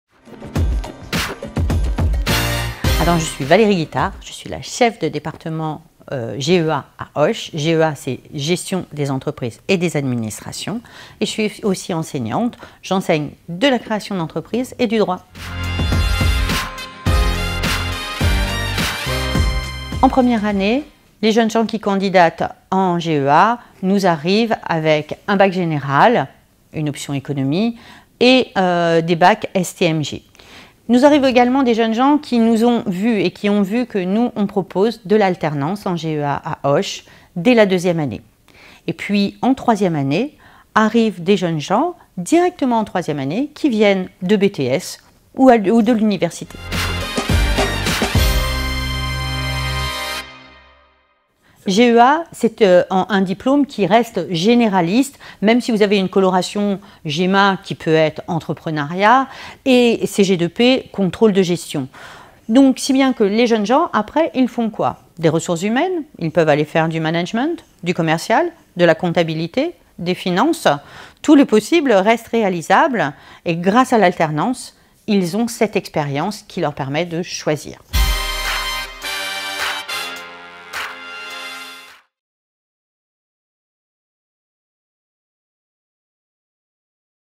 Témoignage enseignante IUT GEA Auch
Témoignage de Valérie, enseignante au BUT Gestion des Entreprises et des Administrations (GEA) à l'IUT Paul Sabatier, campus d'Auch. Valérie vous parle des profils des étudiants en GEA et des débouchés à l'issue de la formation.